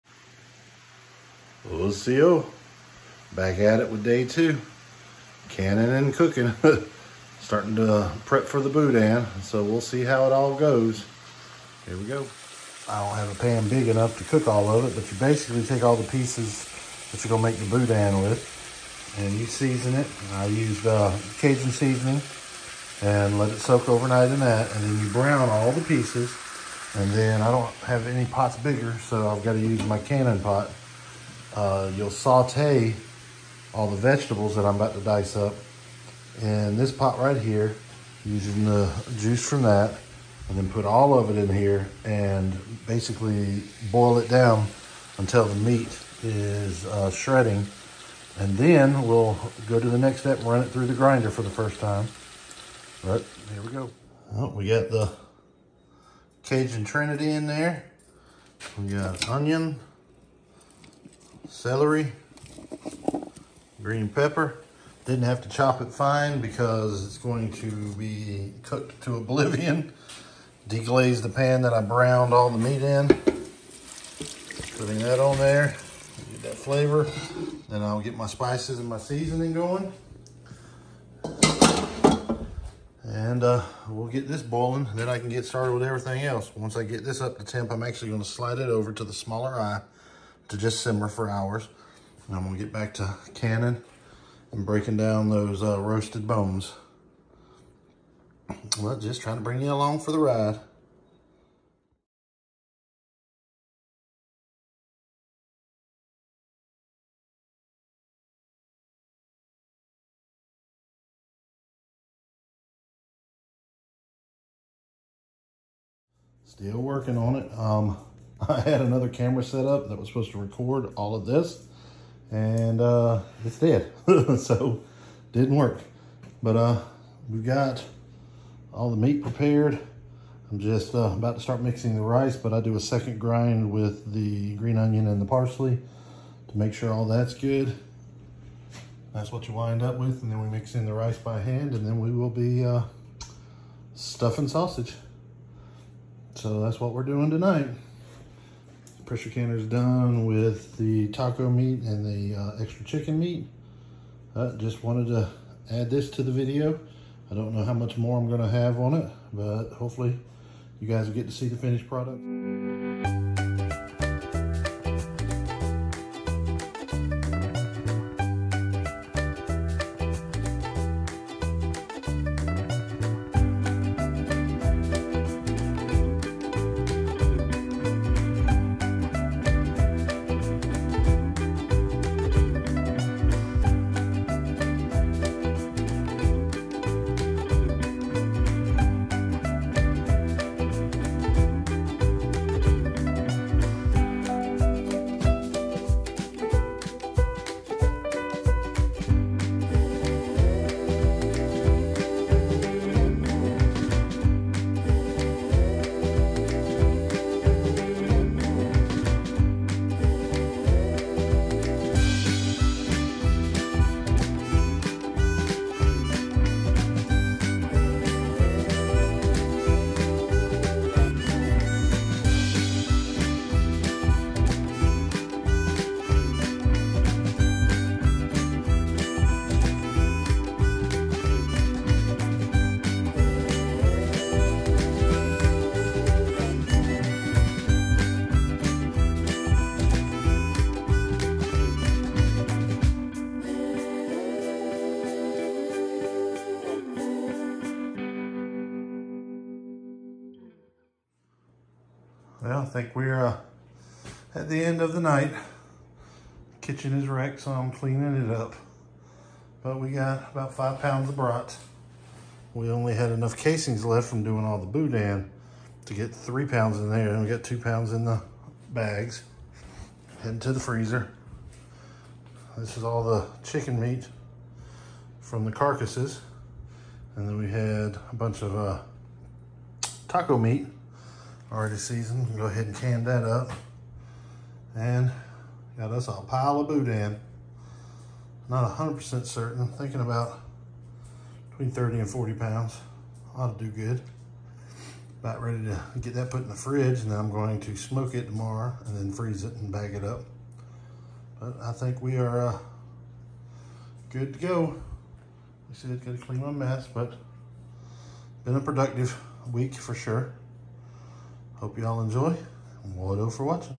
Boudin making today
Love us some boudin and trying to perfect our recipe. We made up about 40lbs of boudin and 5lbs of brats as well as some chicken from the carcasses we made the 2.5 gallons of broth with and some leftover taco meat we had in the fridge.